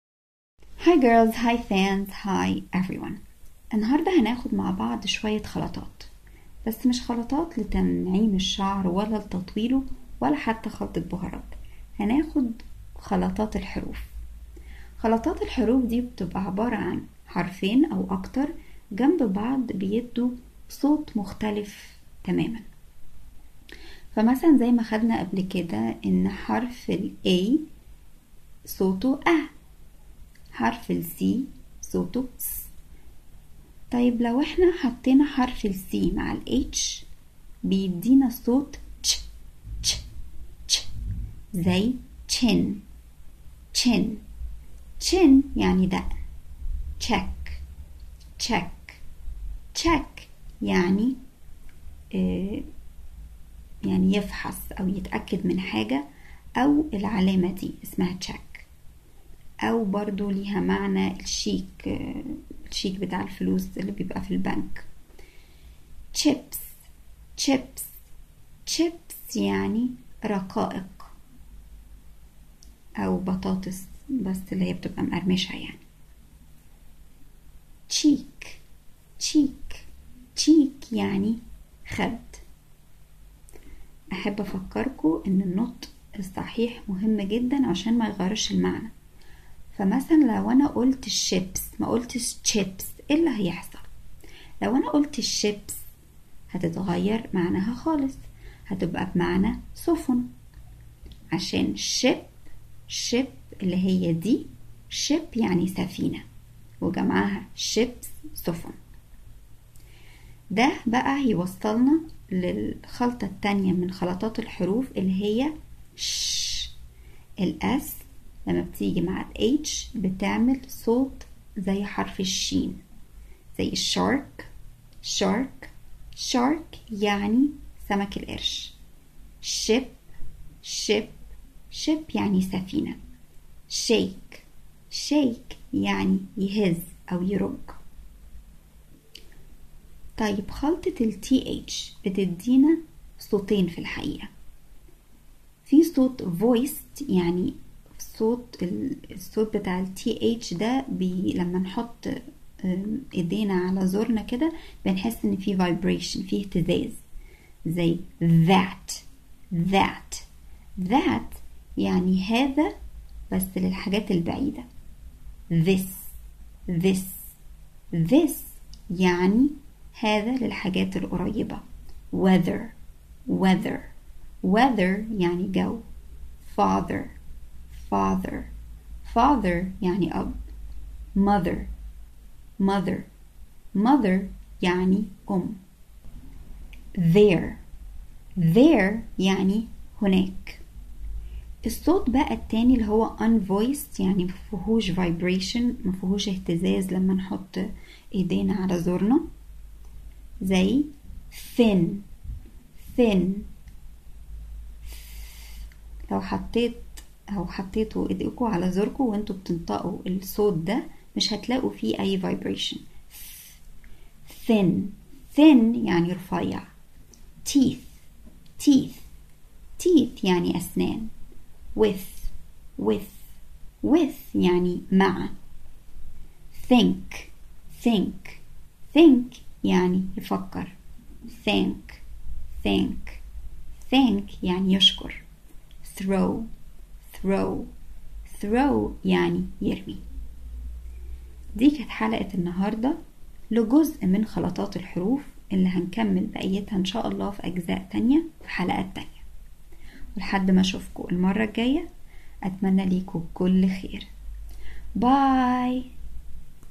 شوية خلطات( ج ١)
فى الفيديو انهاردة هنتعلم مع بعض نطق  حرفين جنب بعض زي الch و الsh و الth
Chin ذقن
Check يفحص
Chips رقائق
Cheek خد
Shark سمك القرش
Ship سفينة
Shake يرج